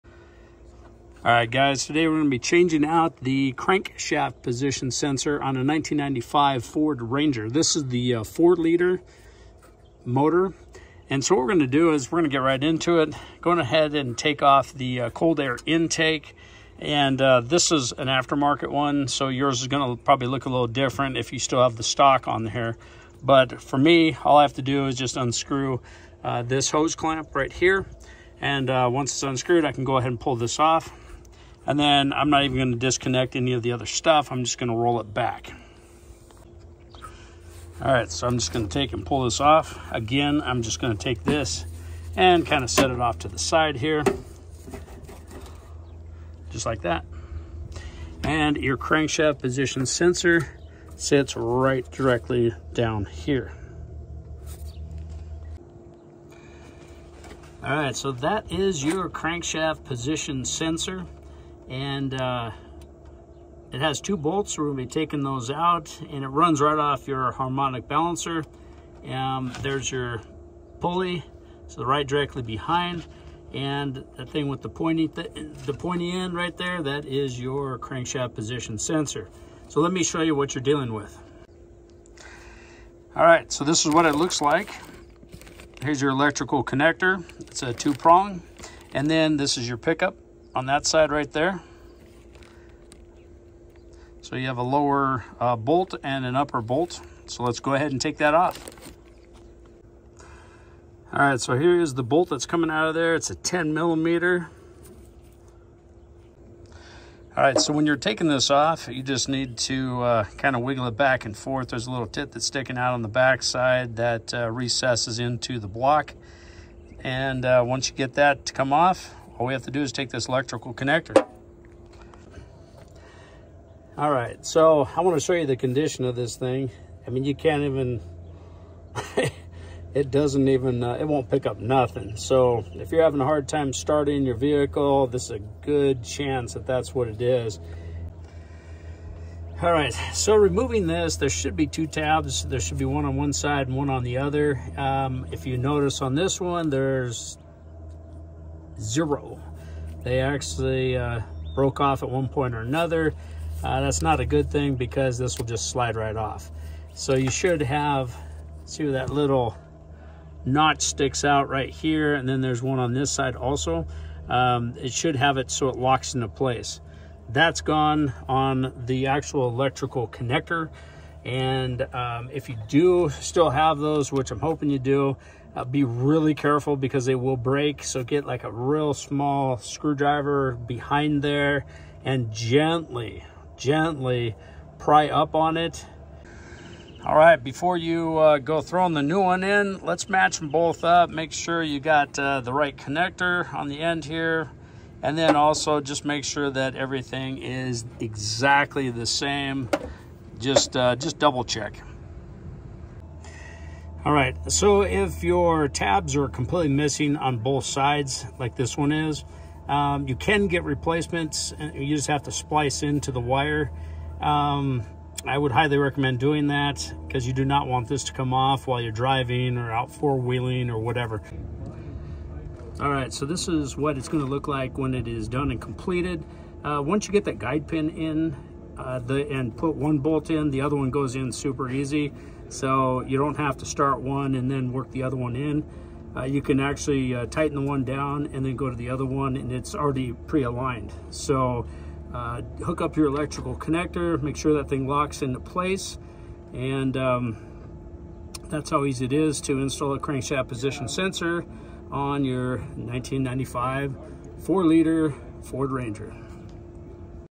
Crankshaft Position Sensor Replacement Ford 4.0 Liter.
This video shows the location, removal and installation of the crankshaft position sensor on a Ford 4.0 engine - Ford Ranger.